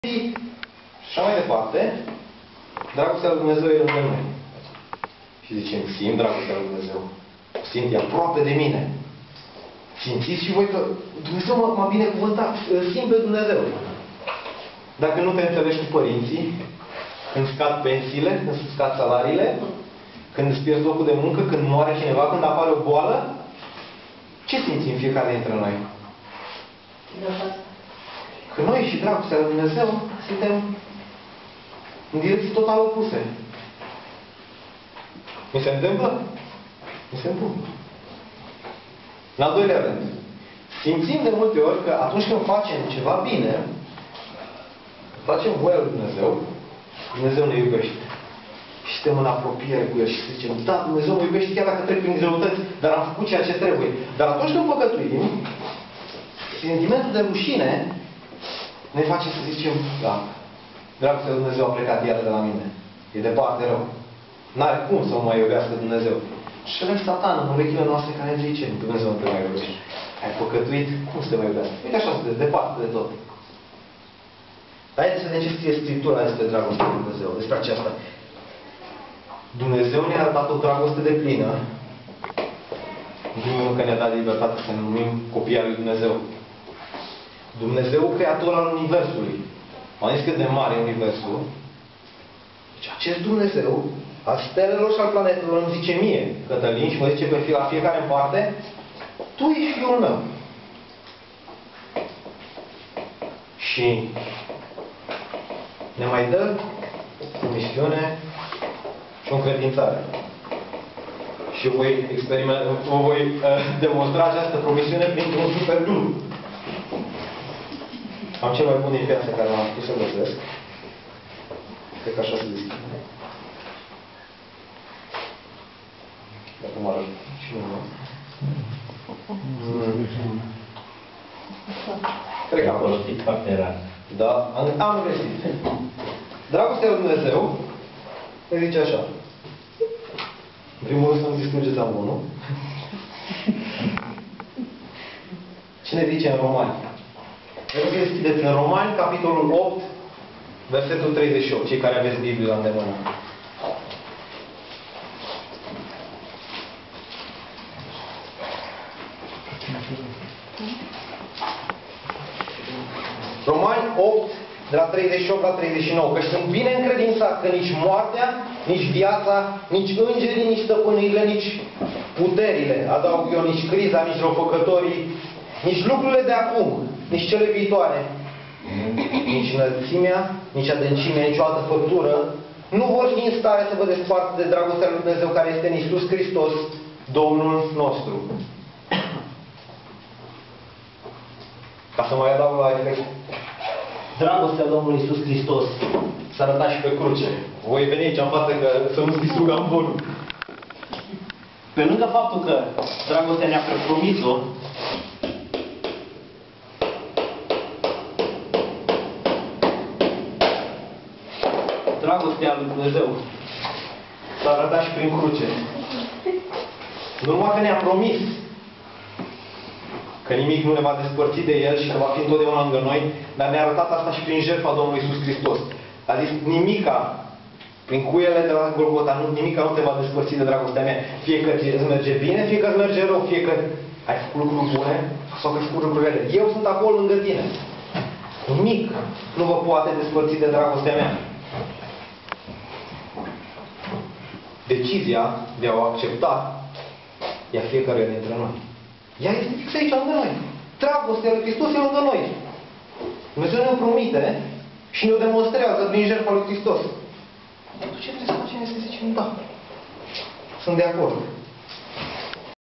Dragostea lui Dumnezeu - Biserica Teliu Brasov
Dragostea lui Dumnezeu - Biserica Teliu Brasov predica Catalin Corches